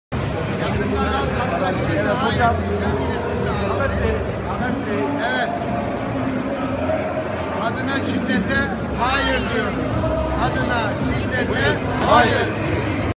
AHMET MARANKİ VE KASTAMONULU AK KADINLAR
Kastamonu Ak Parti Milletvekili Aday Adayı Ahmet Maranki, Kastamonu Ak Parti Kadın Yöneticileri İle "Kadına Şiddete Hayır Dedi"